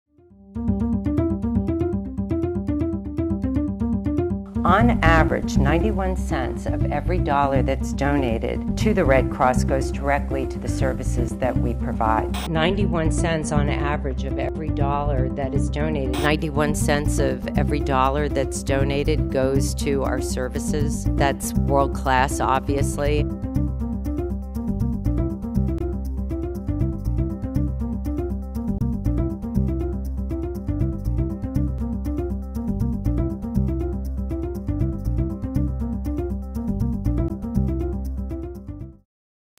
How much of your Red Cross donations go to helping people in need?
The American Red Cross regularly touts how responsible it is with donors’ money. But ProPublica found that the charity spends a far smaller portion of donations on services than it claims.